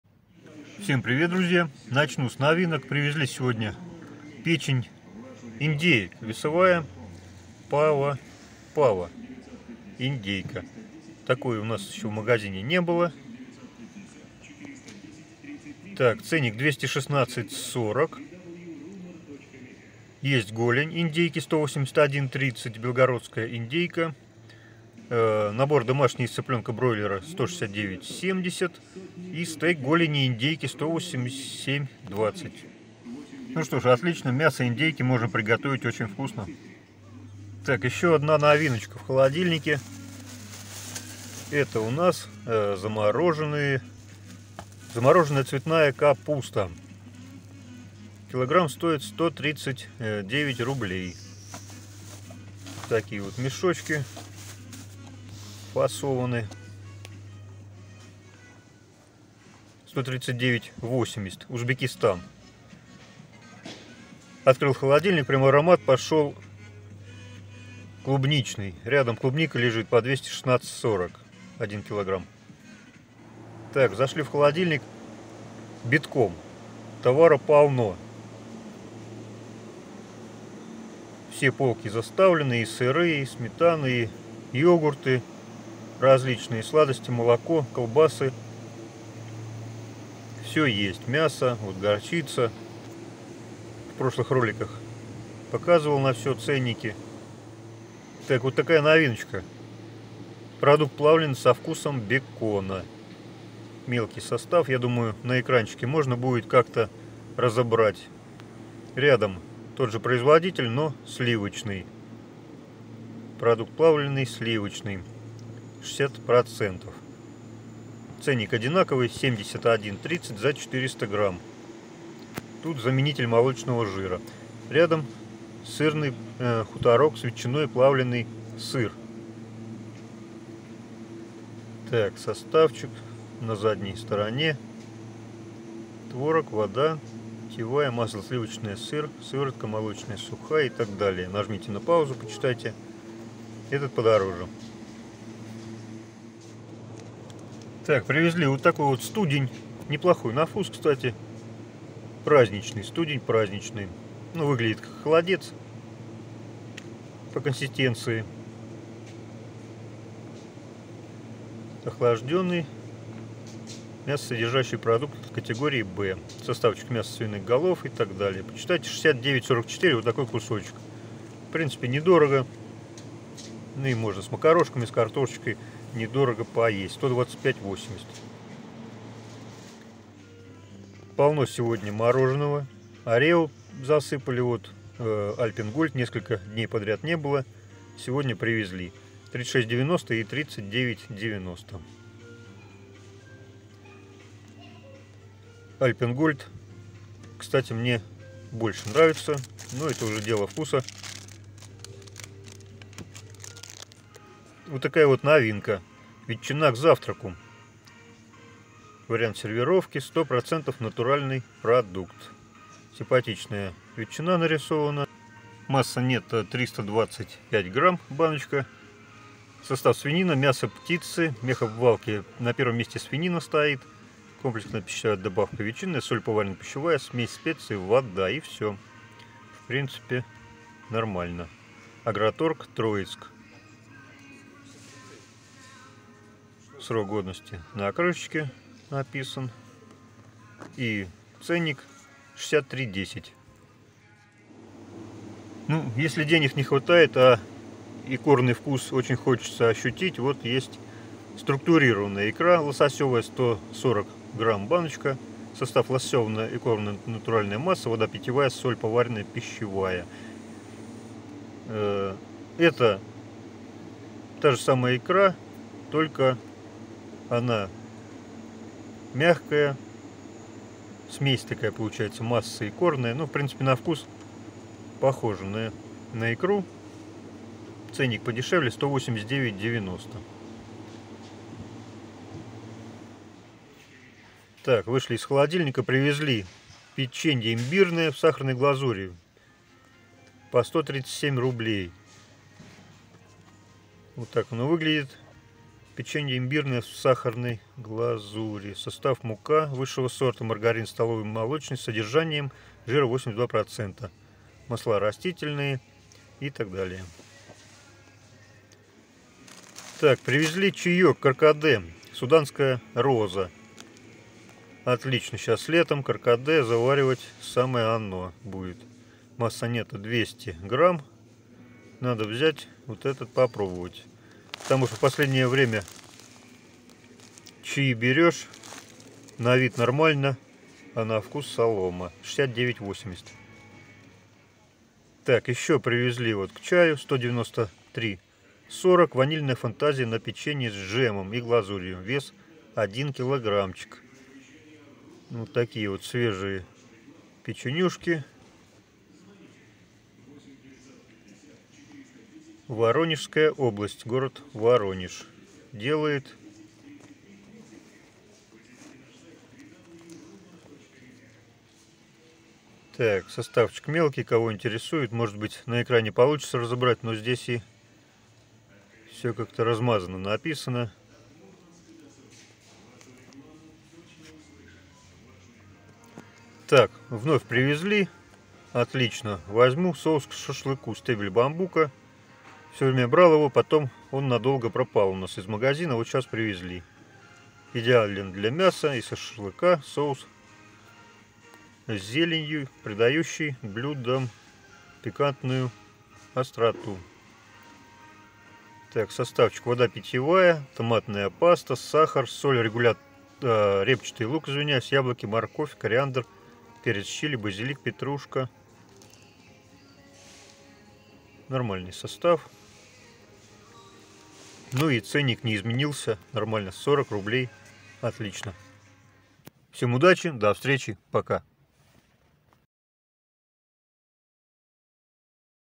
СВЕТОФОР🚦ЭТИХ ПРОДУКТОВ ЗДЕСЬ ЕЩЁ НЕ БЫЛО✅НОВИНКИ👍
ОСОБЕННО СЛОЖНО СЕЙЧАС УСЛЕДИТЬ ЗА ИЗМЕНЕНИЕМ ЦЕН НА ПРОДУКТЫ ПИТАНИЯ. Постараюсь Вам помочь.
Ссылка на мой канал на Яндекс Дзен ,на случай блокировки Ютуба Добро пожаловать,всем рад